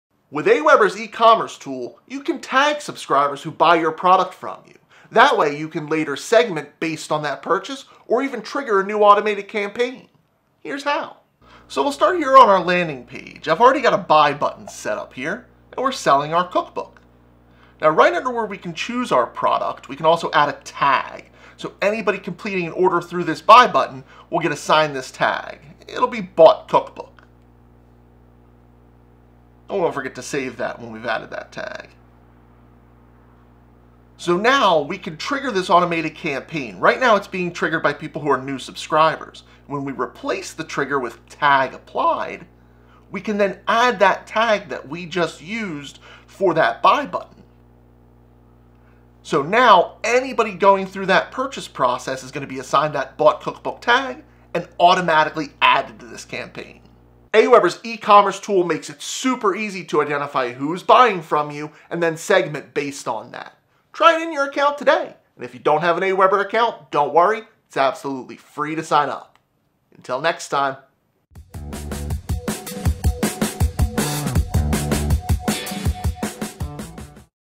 Why should I tag my customers?
When you tag customers who buy through your Landing Page, you can automatically start them on an email campaign offering support, upsells, and more!

Get started with AWeber for free. No credit card. No time restriction. Create a free account now